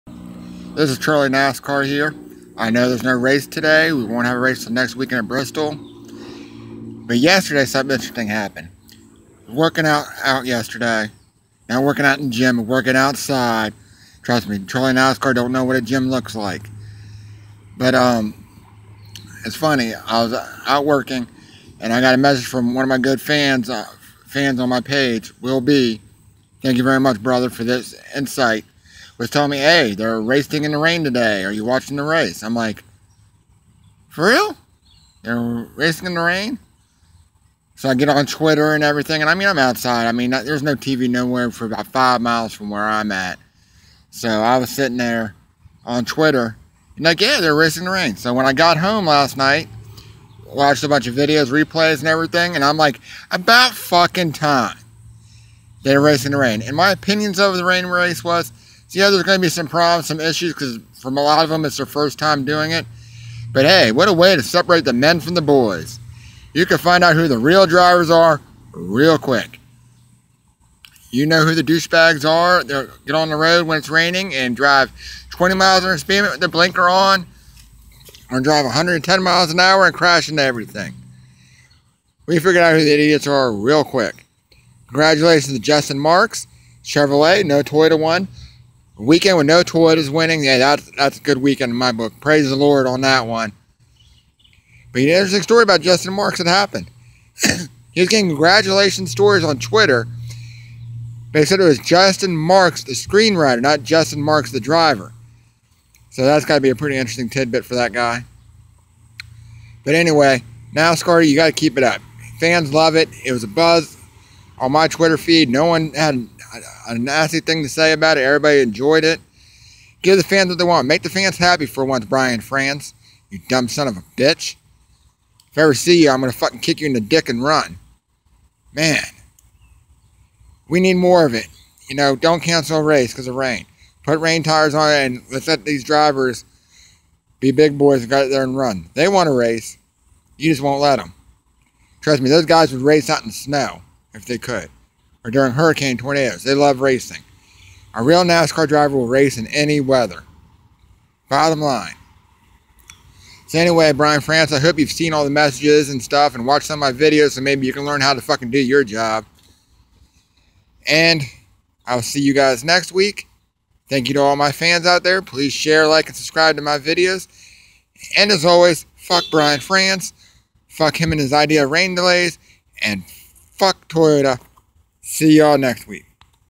Charlie NASCAR talks mid-Ohio rain racing
Charlie NASCAR mid-Ohio rain delays